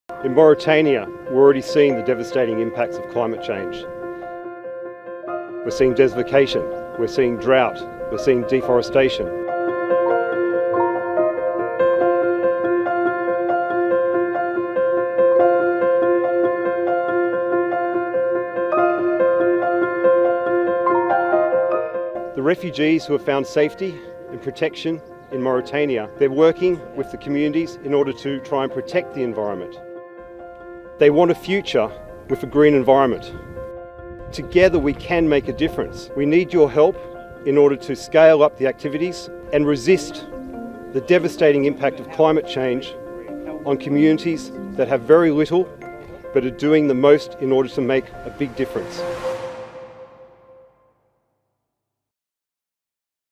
気候変動から難民を守るUNHCRの活動にご支援を
気候変動の最前線から　“気候難民”とは

紛争や暴力だけでなく、干ばつ、水不足等の気候変動の影響も要因となって世界各地で避難を強いられる難民・国内避難民の数が増加しています。そして避難した人々の大多数を受け入れているのもまた、気候危機に対して脆弱な開発途上国です。

いま、行動を起こし気候変動を食い止める。
アフリカ・モザンビークの難民支援の現場から、UNHCR 気候変動特別アドバイザー　アンドリュー・ハーパーのメッセージです。